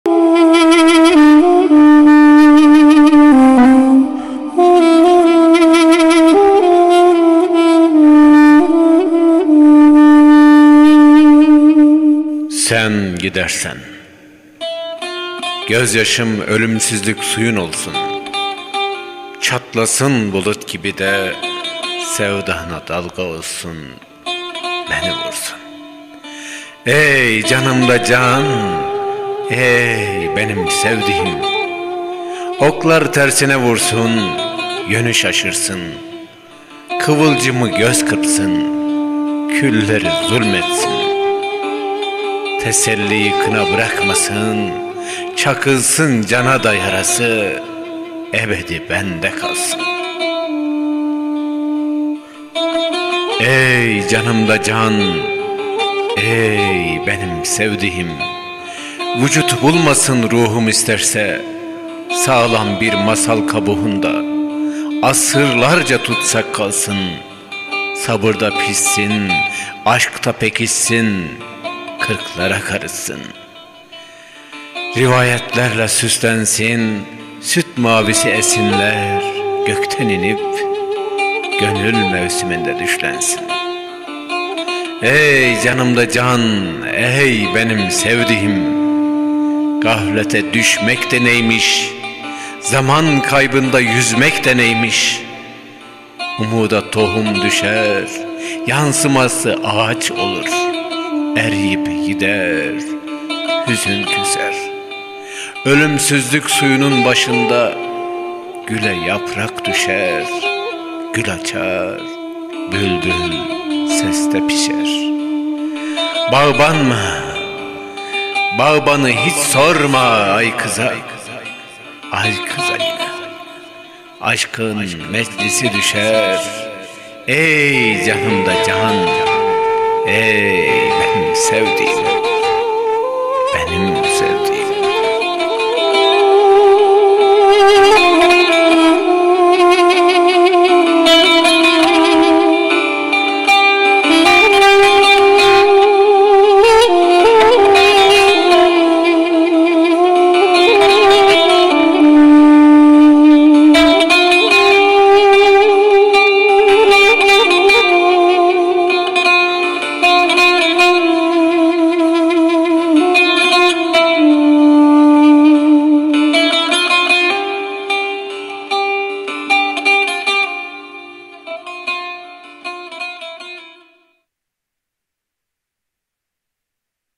"SEN GİDERSEN" Şiir: Leyla Koçak Oruç, Seslendiren: Ali Topsoy, Yapım: Mustafa Karaçiftci
Gözyaşım,
ölümsüzlük suyun olsun.
Çatlasın bulut gibi de,
sevdana dalga olsun,
beni vursun.

Ey, canımda can,
Ey, benim sevdiğim.

Oklar tersine vursun,
yönü şaşırsın.
Kıvılcımı göz kırpsın,
külleri zulmetsin.
Teselliyi kına bırakmasın,
çakılsın cana da,
yarası,
ebedi bende kalsın.

Ey, canımda can,
Ey, benim sevdiğim.

Vücut bulmasın,
ruhum isterse.
Sağlam bir masal kabuğunda,
asırlarca tutsak kalsın.
Sabırda pişsin,
aşkta pekişsin,
Kırklara karışsın.

Rivayetlerle süslensin,
süt mavisi esinler,
gökten inip,
gönül mavisinde düşlensin.

Ey, canımda can,
Ey, benim sevdiğim.

Gaflete düşmek de neymiş?
Zaman kaybında yüzmek de neymiş?

Umuda tohum düşer,
yansıması ağaç olur.
Eriyip gider,
hüzün küser.
Ölümsüzlük suyunun başında,
güle yaprak düşer,
gül açar,
bülbül seste pişer.

Bağban mı?
Bağbanı hiç sorma...
Aykız'a,
Aykız'a yine,
Aşk'ın meclisi düşer.

Ey, canımda can,
Ey, benim sevdiğim.

Leyla Koçak Oruç